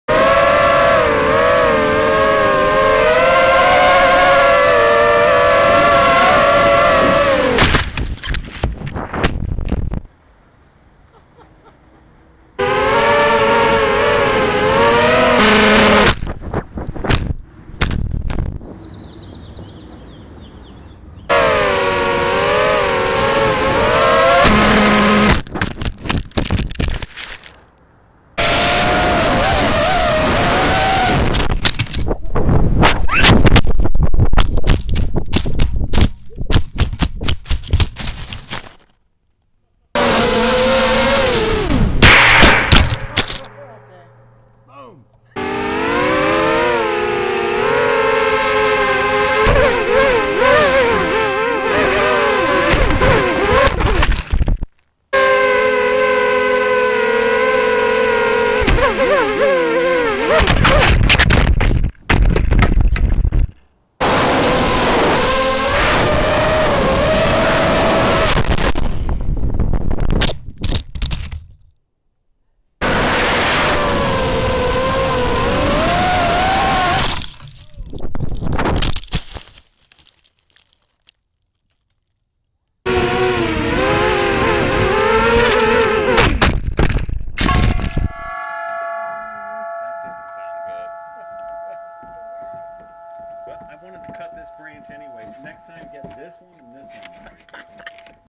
10 Crashes
Drone hits trees, building, chimney, guy wire, propane tank, trash can, ground, and more trees